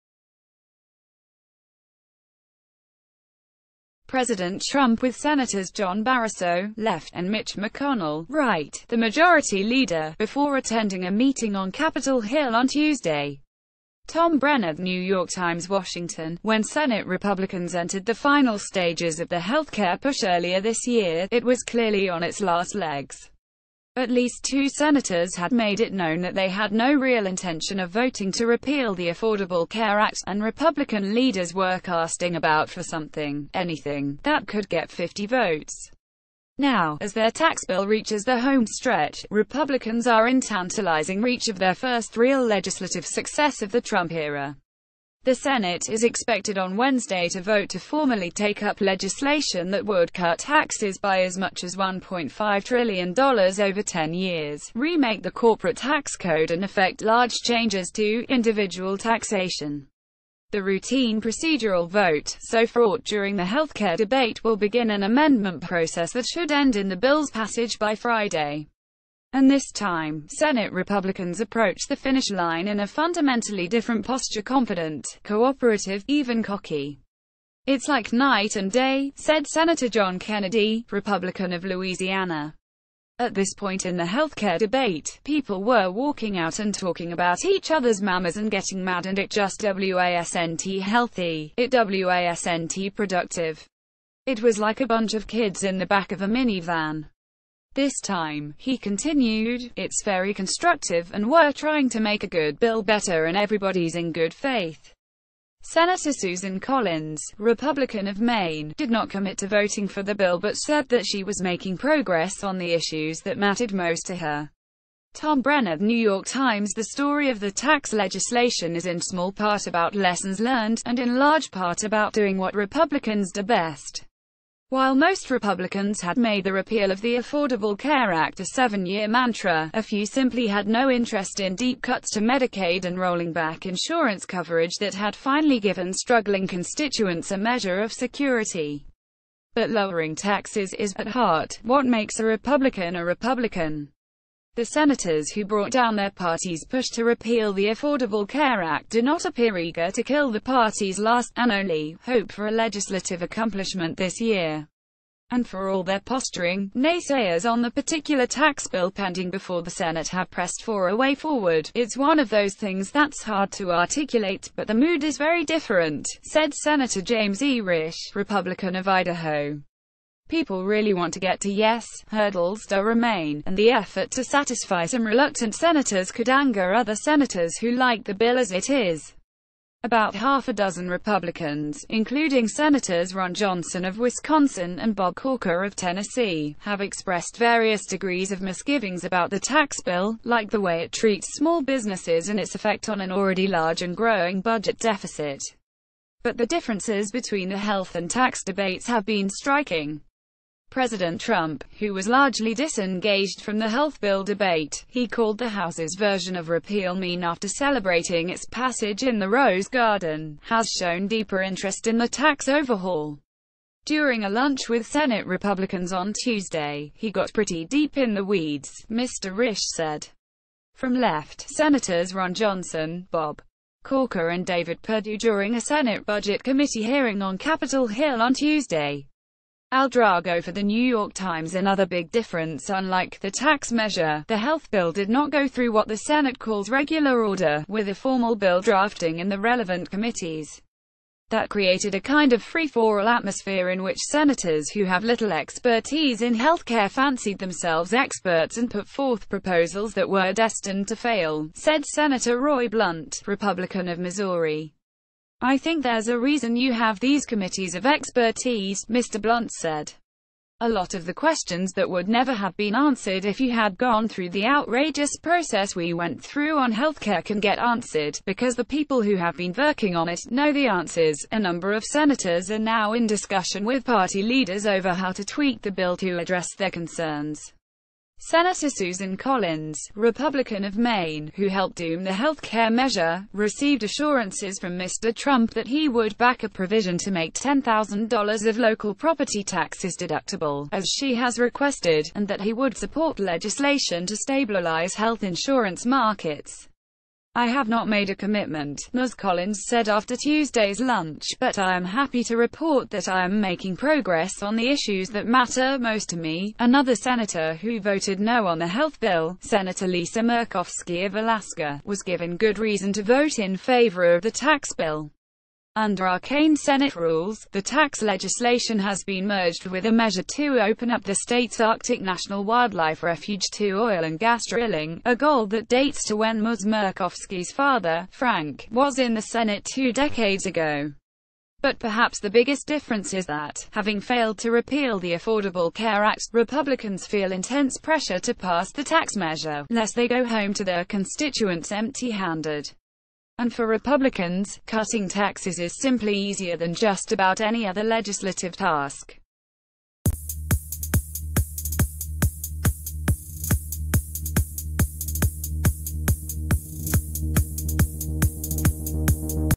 Republicans, entering homestretch on tax cuts, are calm and cooperative | News TV
President Trump with Senators John Barrasso, left, and Mitch McConnell, right, the majority leader, before attending a meeting on Capitol Hill on Tuesday. Tom BrennerThe New York Times WASHINGTON — Wh...